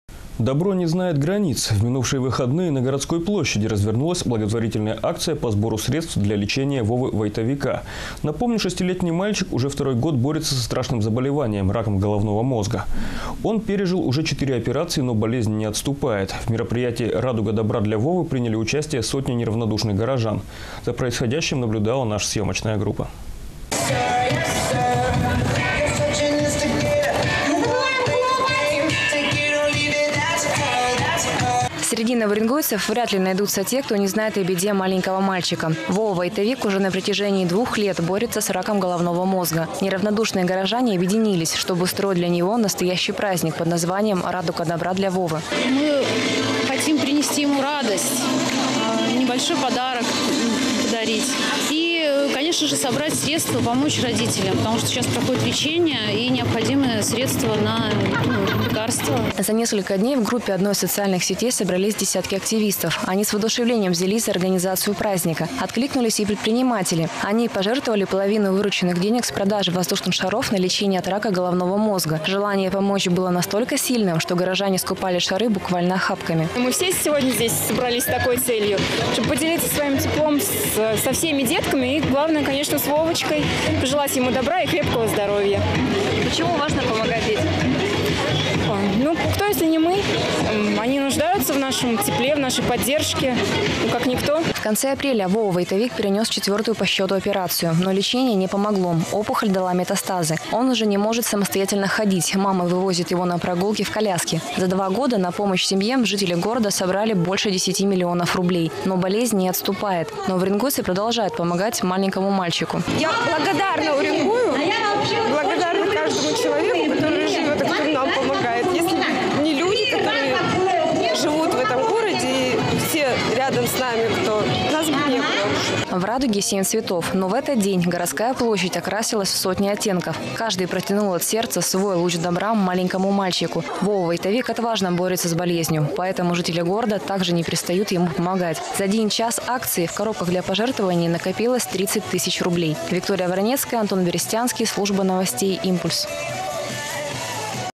Жители города продолжают собирать средства на лечение Вовы Войтовика.
Добро не знает границ. В минувшие выходные на городской площади развернулась благотворительная акция по сбору средств для лечения Вовы Войтовика. Напомню, шестилетний мальчик уже второй год борется со страшным заболеванием – раком головного мозга. Он пЕрежил уже четыре операции, но болезнь не отступает. В мероприятии «Радуга добра для Вовы» приняли участие сотни неравнодушных горожан. За происходящим наблюдала наша съемочная группа.